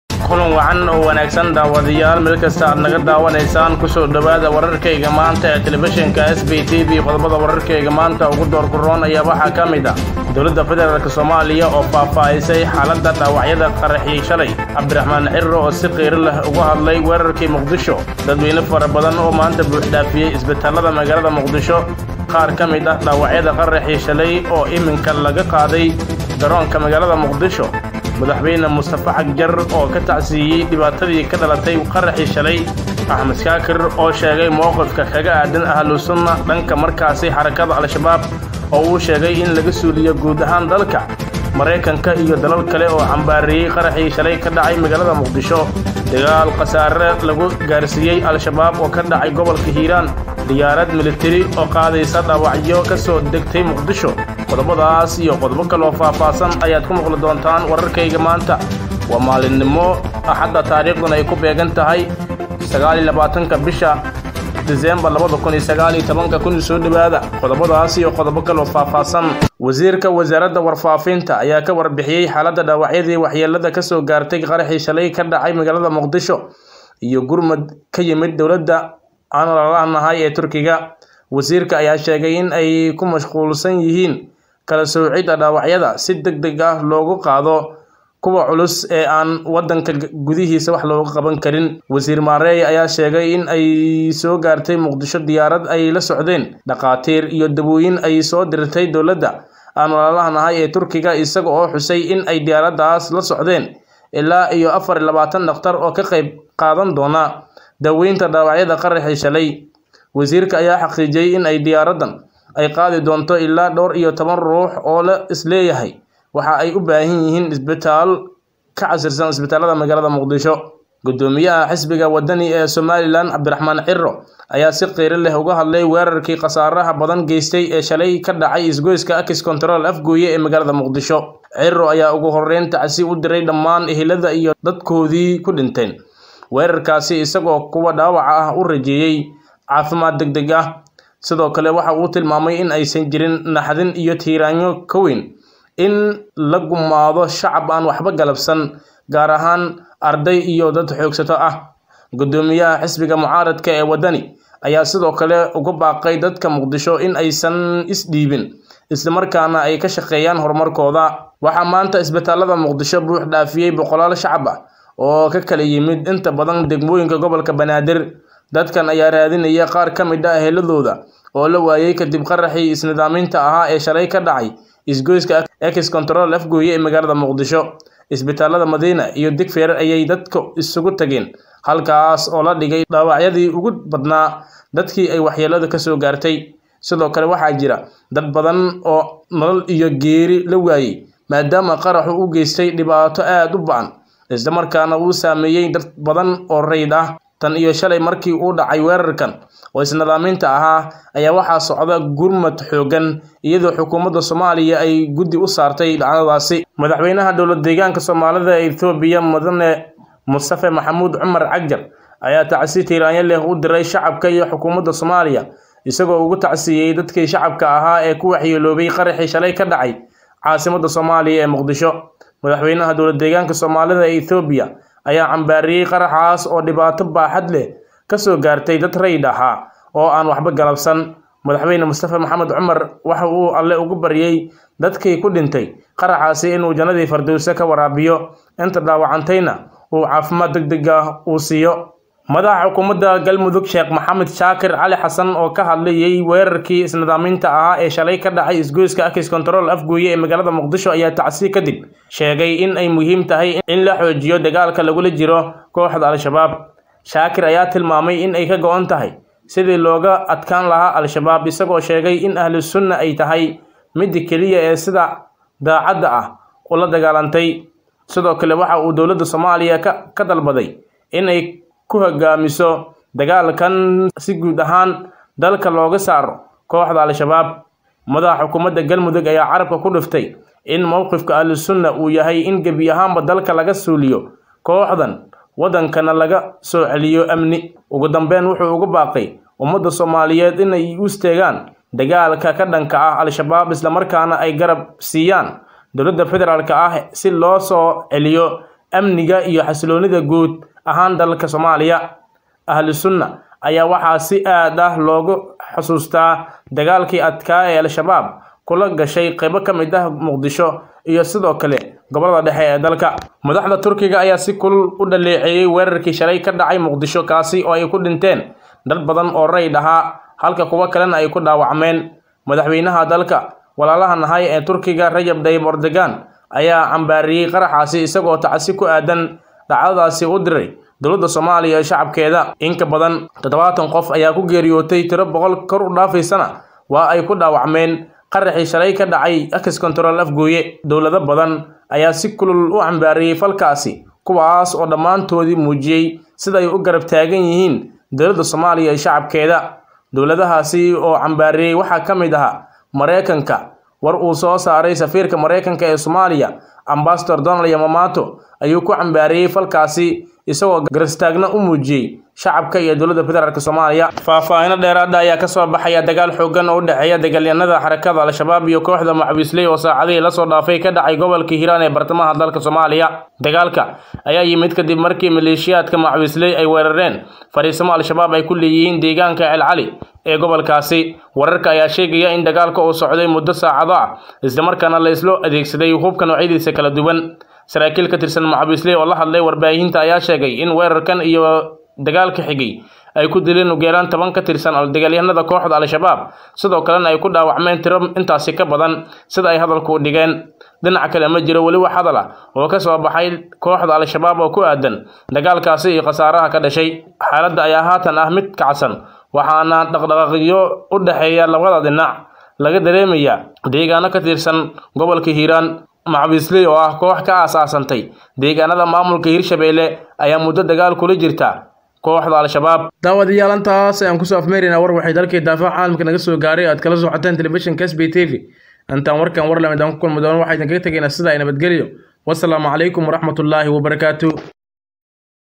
WARKA SBTV29-12-19 DIYAARAD MILITIRI QAADAY DHAAWACYO,CAGJAR OO KA TACSIYEEY,DAGAAL KA DHACAY HIIRAA
WARKA SBTV29-12-19 DIYAARAD MILITIRI QAADAY DHAAWACYO,CAGJAR OO KA TACSIYEEY,DAGAAL KA DHACAY HIIRAAN.